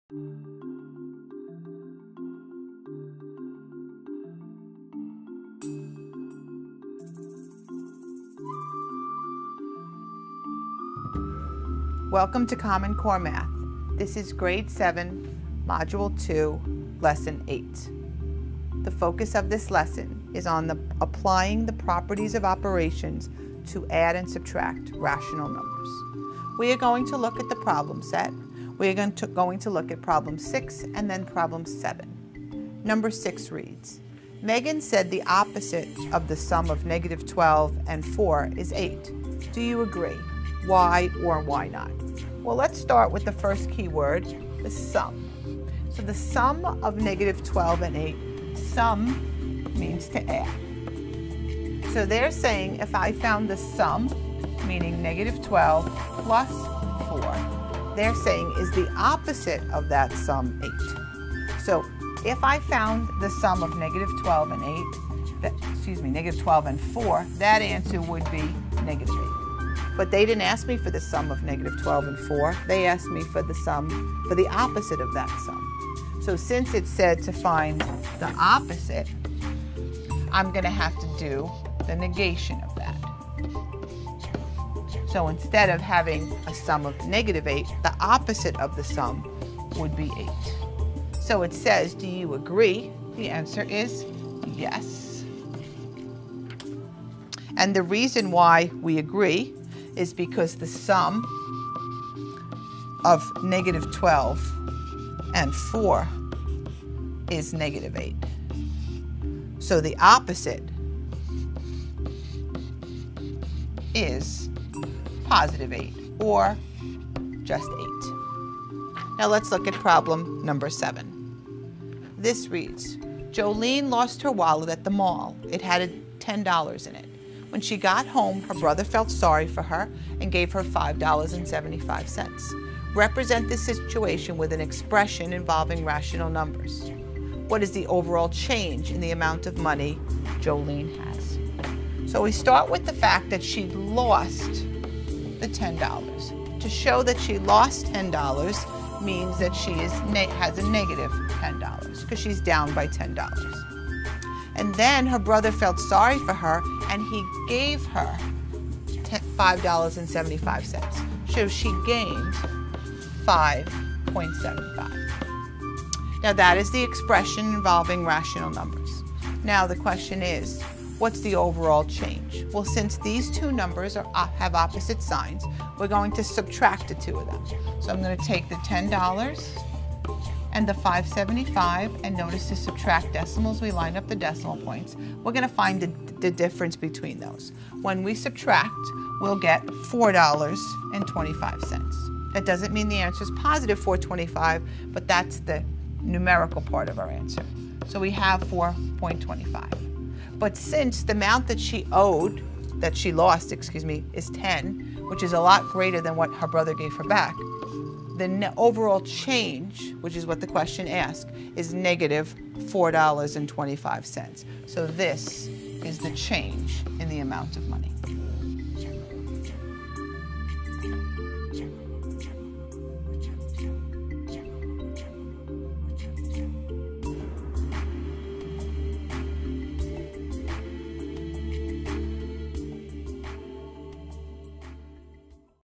Grade 7 Module 2 Lesson 8
Applying the properties of operations to add and subtract rational numbers.